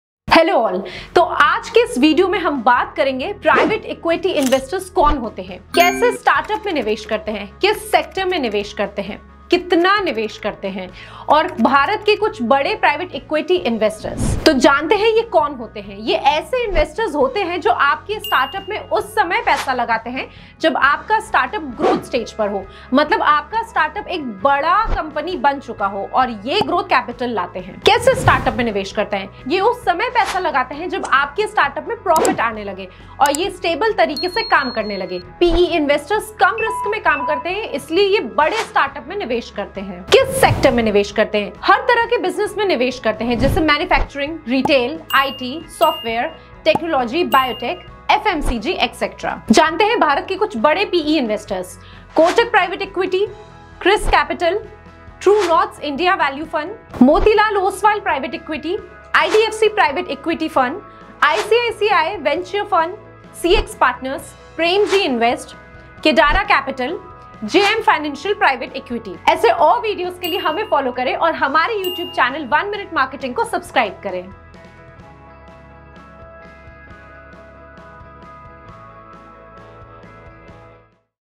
Private Equity PE investors कौन होते है? | One Minute Marketing
Private Equity (PE) investors कौन होते है ?2021
तो बात करेंगे इस ब्लॉग में –
• Private Equity (PE) investors  कौन होते है ?
• Private Equity (PE) investors  कैसे स्टार्टअप में निवेश करते है ?
• Private Equity (PE) investors   किस sector में निवेश करते है ?
• Private Equity (PE) investors  कितना निवेश करते है ?
• भारत के कुछ  बड़े Private Equity (PE) investors

• Kotak Private Equity
• Chrys capital
• True North’sIndia Valuefund
• Motilal Oswal Private Equity
• IDFC Private Equity Fund
• ICICI Venture Fund
• CX Partners
• JM Financial Private Equity